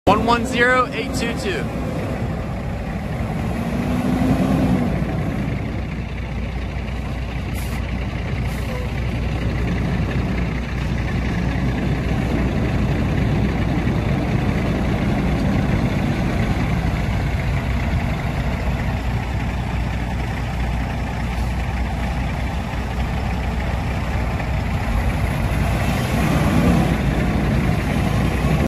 2010 INTERNATIONAL PROSTAR For Sale
MIDRISE SLEEPER, AIR RIDE SUPENSION, MAXX FORCE 13' DIESEL MOTOR A/T, VIN-3HSDBSJR2AN279228, (506,732 MILES SHOWING)

 [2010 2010 PROSTAR T/A ROAD TRACTOR]